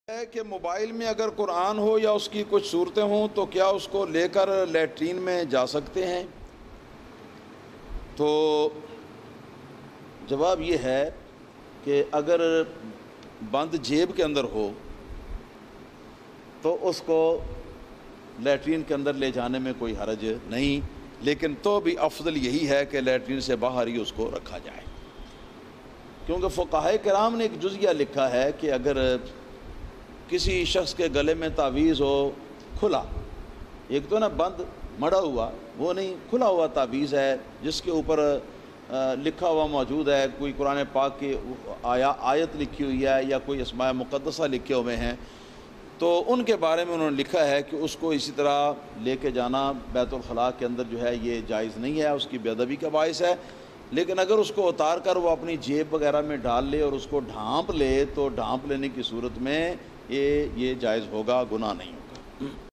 Mobile Main Quran Paak Ho To Kya Mobile K Sath Bathroom Main Jaa Skte Hain Ya nahi
Mobile Main Quran or Bathroom

Muftiye Azam Pakistan Mufti Muhammad Ashraf-ul-Qadri Muhaddas Naik Abadi Sajjada Nashin Murarian Sharif(Naik Abad)
Bani Al-Jamia -tul-Ashrfia Ali Masjid Markazi Gujrat Pakistan